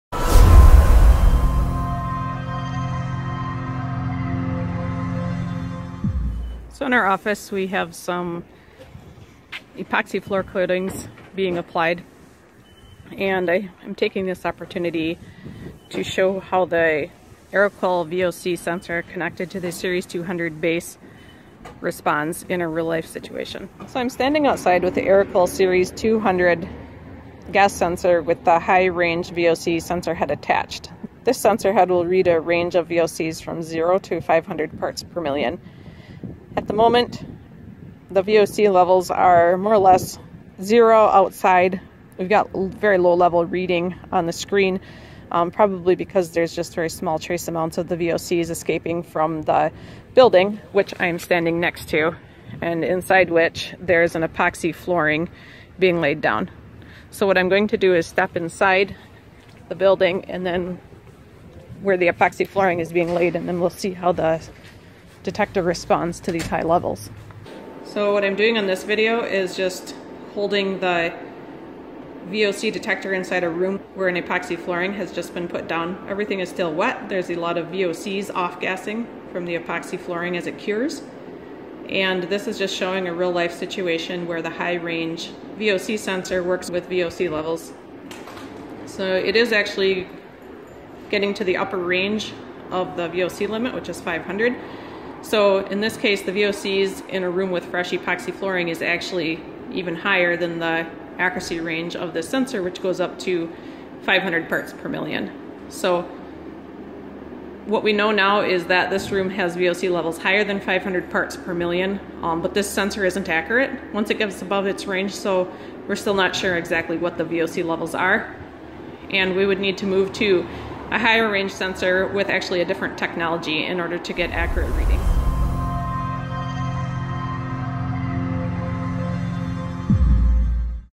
Aeroqual GSS VOC Sensor with 0-500 ppm Range Measuring VOC Levels in a Room with New Epoxy Floor
Here we provide a real-world example of the Aeroqual Series 500 Gas Detector. This video demonstrates the use and range limitations of the Aeroqual Series 500 high-range GSS sensor (range of 0-500 ppm). The scenario is an epoxy floor recently laid inside a work-space in a
warehouse. You can see the low level readings from the fumes outside of the immediate
area where the floor had been laid. In the video, you notice the jump in readings while
inside the building. This is due to the 30 second intervals the the machine updates its
readings. The high-level VOC sensor will have readings that move upward steadily and
will not fluctuate very much. The high range Aeroqual gas detector sensor cartridge, part
number VP, is designed to measure VOCs in the range of 0 to 500 parts per million
(ppm). The resolution and sensitivity of the high-range sensor are lower than the low-
range sensor, which is what causes the steadier readings and increased accuracy at high
VOC levels until the readings exceed 500 ppm.

The high-level VP Aeroqual VOC cartridge sensor head is compatible with the Aeroqual
Series 200, Series 300, and Series 500 gas monitor bases.